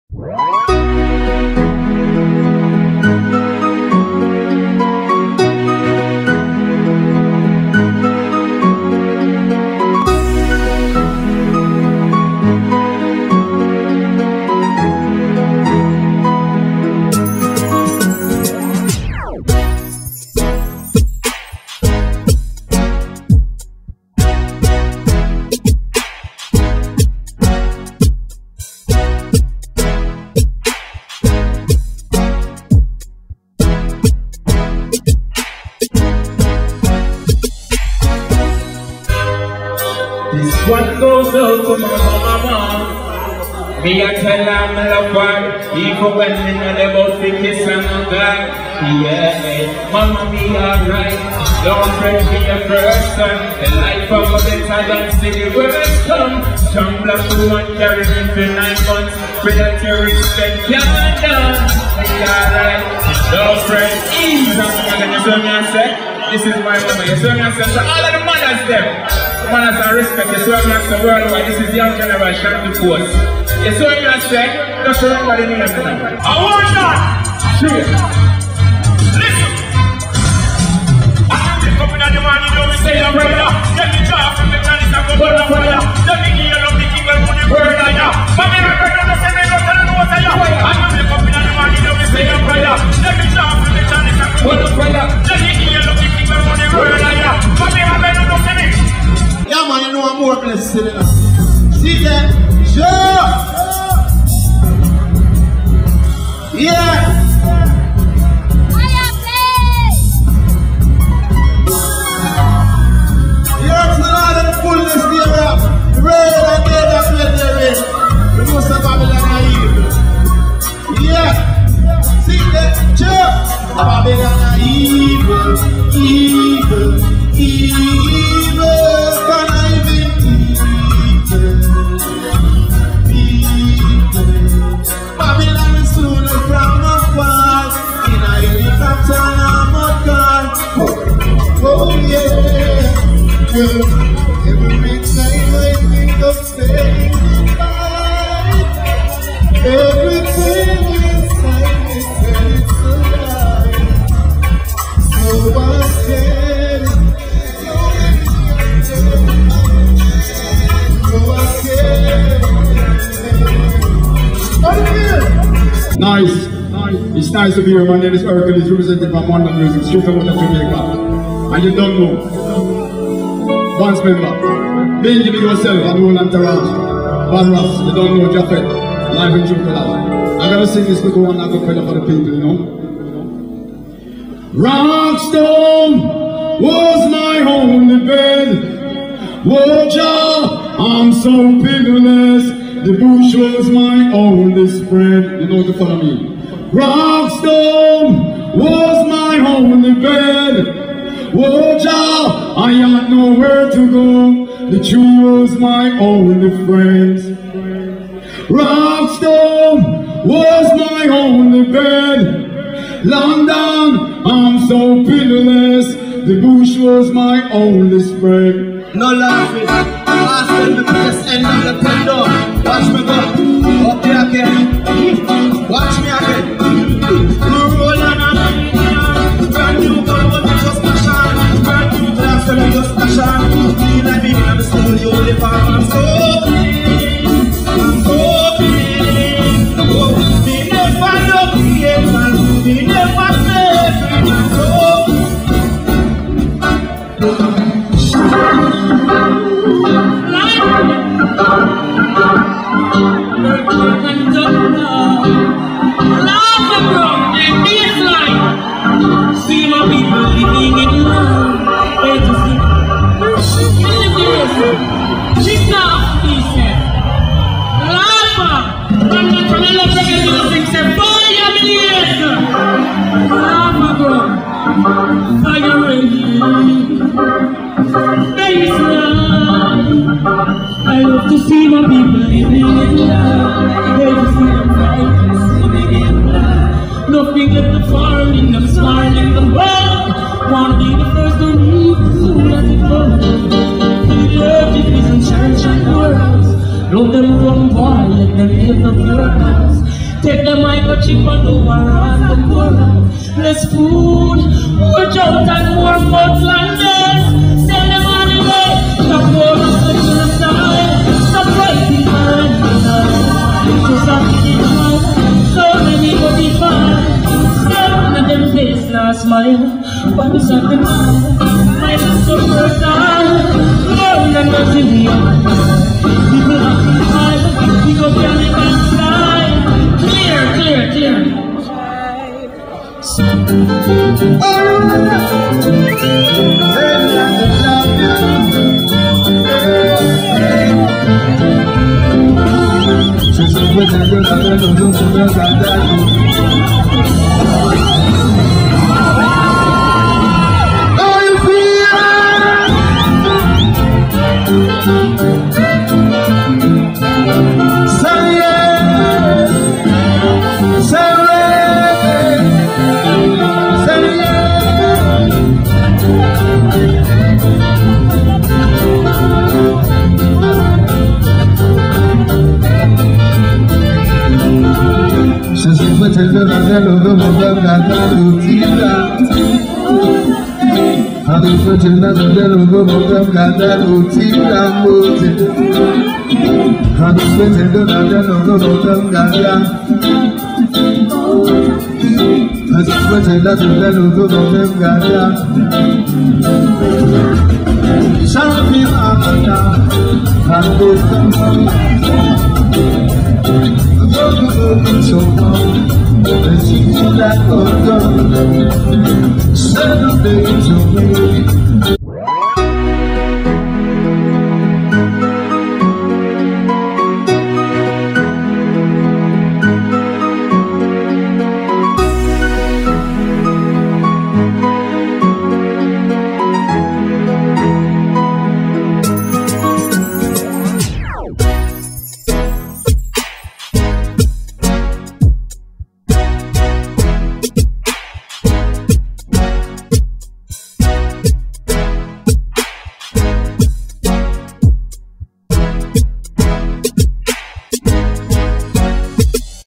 POSITIVE VIBRATIONS 2014 @ THE CORONET
ENTERTAINMENT LONDON WAS AT THE POSITIVE VIBRATIONS 2014 EVENT @ THE CORONET. SUNDAY 18TH MAY,E.L CAUGHT SOME GREAT VIDEO CLIPS OF SOME OF THE ARTIST'S BEST MOMENTS WHO FEATURED AT THIS EVENT SUCH AS SHANTI FORCE - FOWLIE DON - HYA P - BADRASS - HERCULES - I WAYNE & LUCIANO